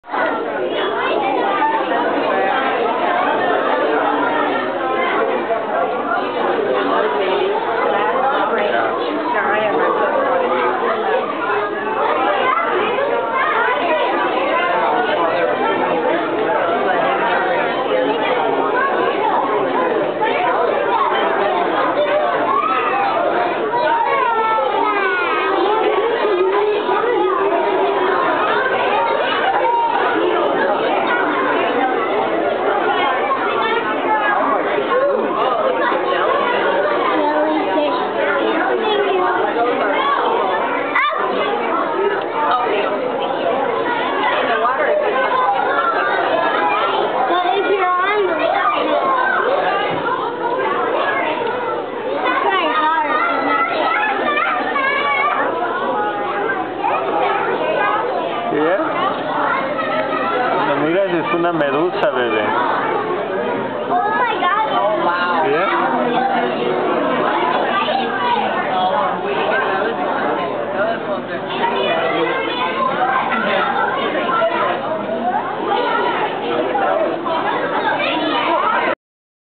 Birch Aquarium With Amelie 2
AAA Beautiful Ghost Jellyfish.., These and the Sharks are my favorite..